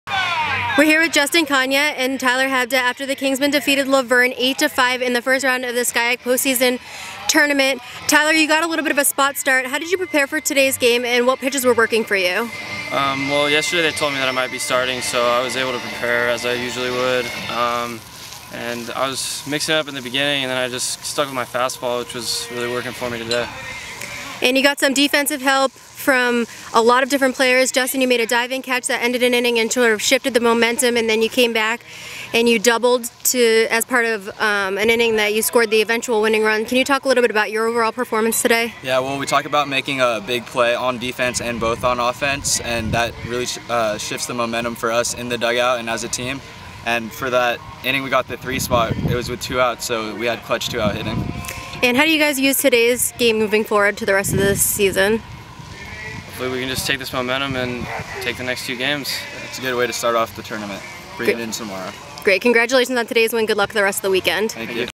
Baseball May 2 SCIAC Post Season Tournament LV
Justin Cana and Tyler Hebda talk about the 8-5 Cal Lutheran victory over La Verne in the first round of the 2014 SCIAC Post Season Tournament hosted by Cal Lutheran today, Friday, May 2.